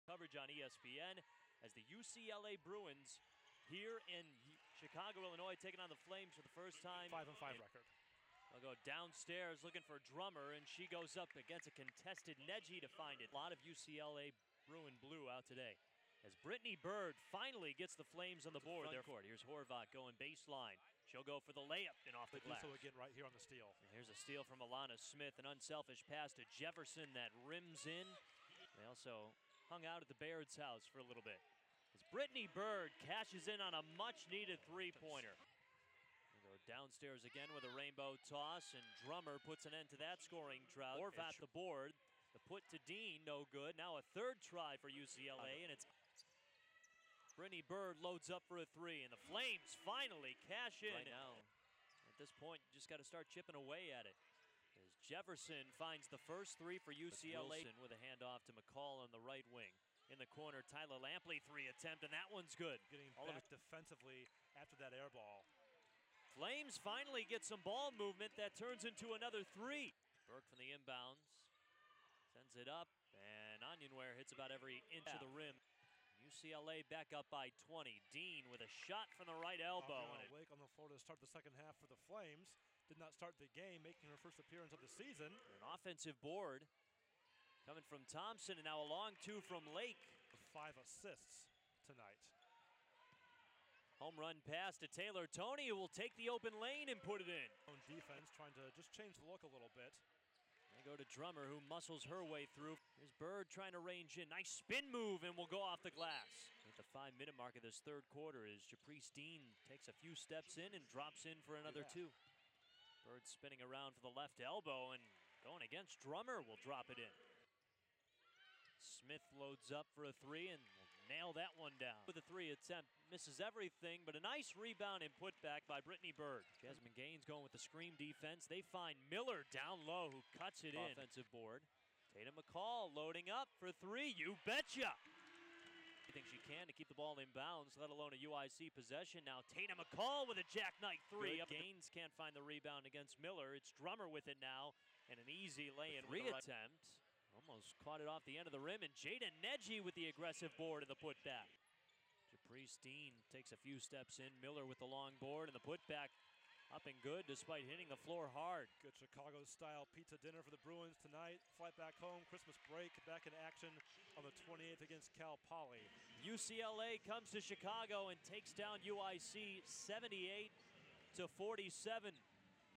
UIC WBB Highlights vs. UCLA 12-19-18
UIC women's basketball highlights vs. UCLA
Final Score: UCLA 78, UIC 47